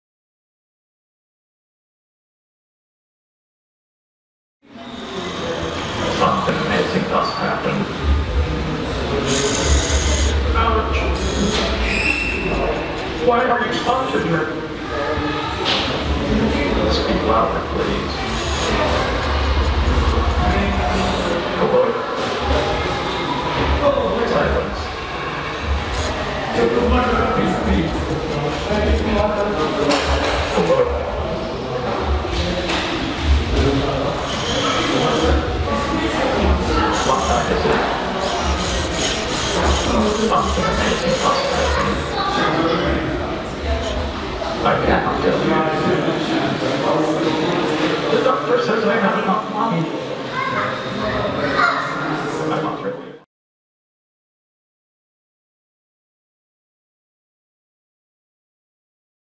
Cafe Neu Romance 2014: Fernando Orellana: Dramatis Personae
Cafe Neu Romance is the World's first international robot performance festival. It took place for the third time at Galerie NTK, Balling Hall at NTK & Institute of Intermedia at CTU in Prague November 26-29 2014. Cafe Neu Romance 2014 was organized by the Danish robot event & marketing company Vive Les Robots! and co-organized by Galerie NTK of the National Technical Library.

In this video you can watch Fernando Orellana's robotic art installation Dramatis Personae in action.